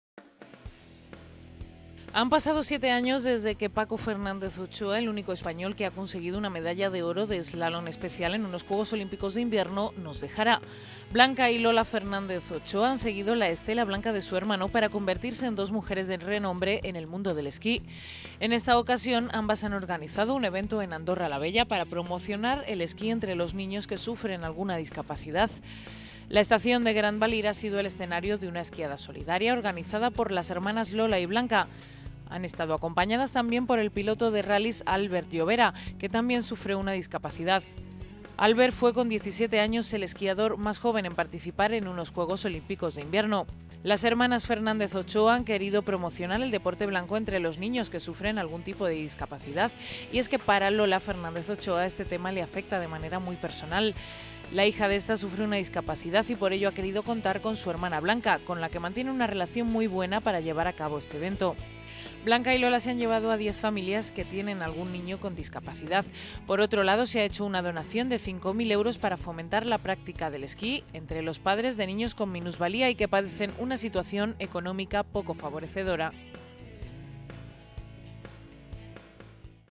Las Hermanas Ochoa apuestan por el esquí solidario
Blanca y Lola Fernández Ochoa han seguido la estela blanca de su hermano para convertirse en dos mujeres de renombre en el mundo del esquí. En esta ocasión, ambas han organizado un evento en Andorra la Vella para promocionar el esquí entre los niños que sufren alguna discapacidad.